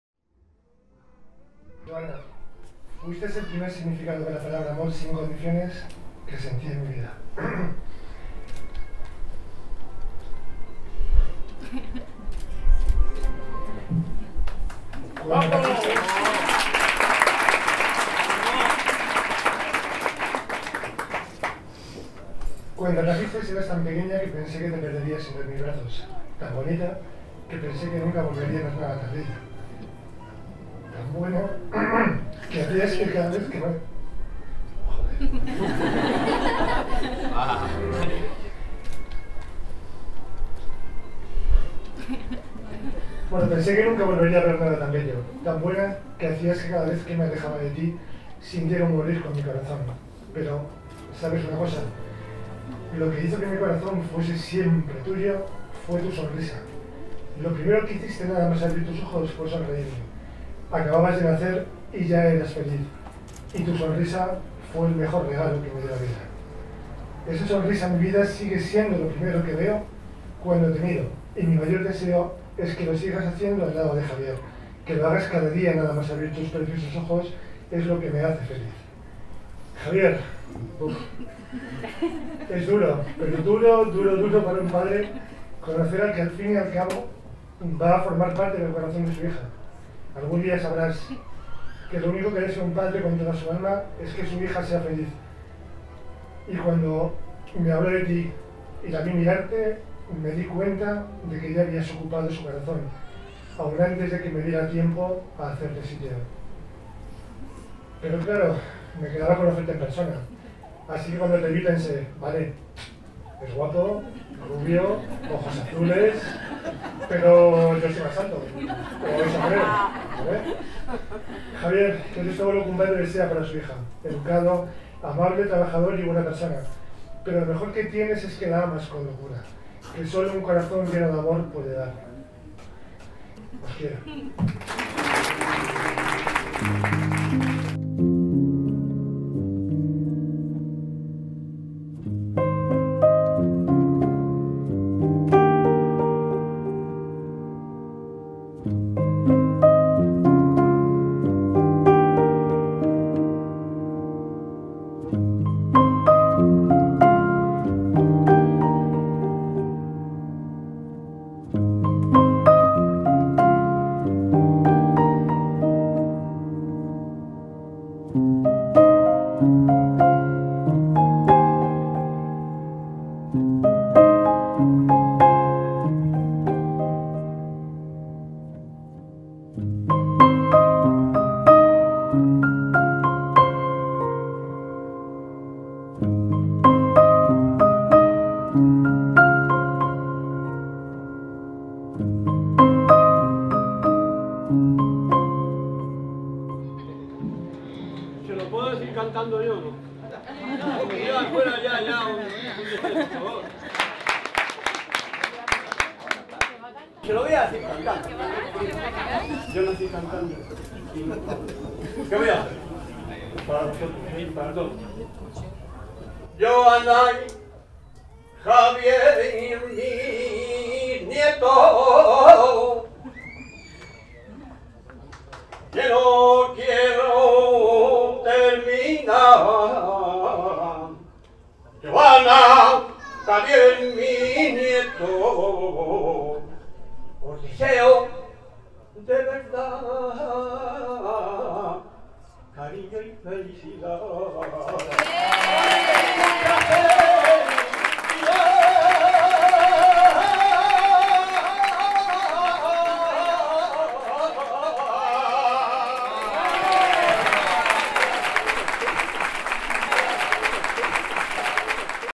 VIDEO DE BODA JOANNA+JAVIER TRAILER -Videos de boda Valencia by Pilar Prada-
Una pareja encantadora con la que tuvimos la suerte de estar en su reportaje de boda. Fue una boda muy emotiva realizada en Jardines Azahar, en Valencia.
pilarprada.com Videos de Boda en Valencia y Castellón